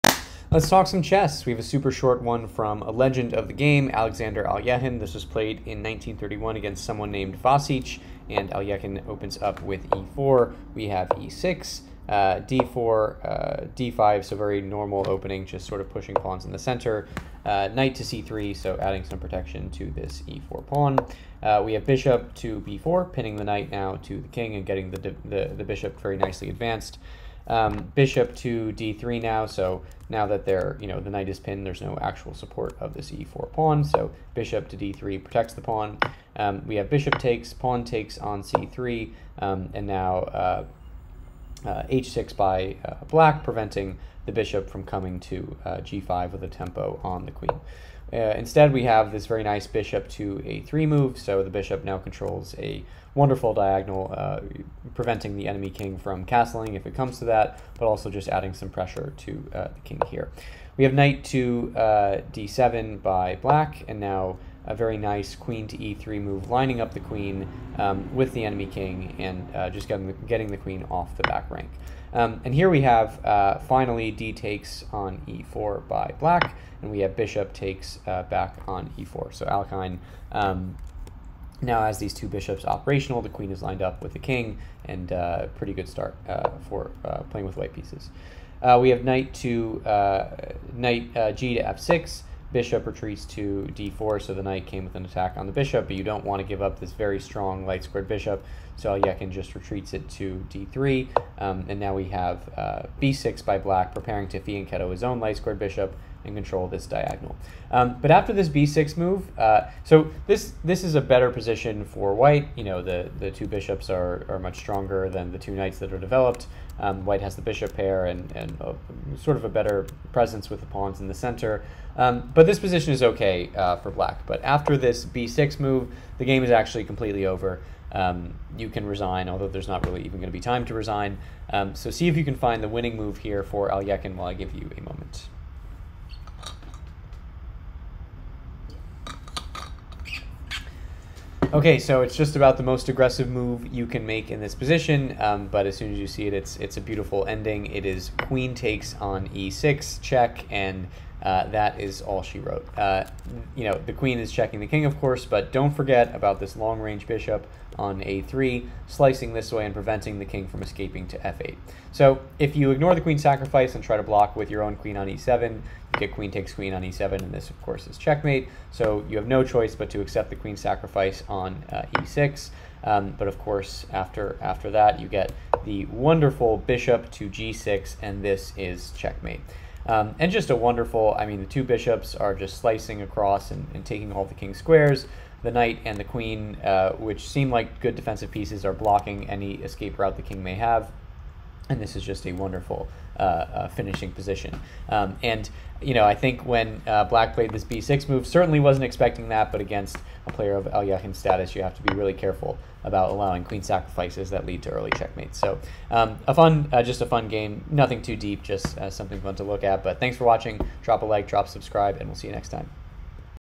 Alekhine Strikes Like Lightning (Banja Luka — 1931)
Thanks for talking chess with me! This was part of a simul, and Alekhine made sure to dispatch of his opponent quickly. This is the lichess analysis board.

[Event "Simul, 35b"]
[Site "Banja Luka YUG"]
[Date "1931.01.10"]
[EventDate "?"]
[Round "?"]
[Result "1-0"]
[White "Alexander Alekhine"]
[Black "Vasic"]
[ECO "C15"]
[WhiteElo "?"]
[BlackElo "?"]
[PlyCount "21"]

1. e4 e6 2. d4 d5 3. Nc3 Bb4 4. Bd3 Bxc3+ 5. bxc3 h6 6. Ba3
Nd7 7. Qe2 dxe4 8. Bxe4 Ngf6 9. Bd3 b6 10. Qxe6+ fxe6 11. Bg6#
1-0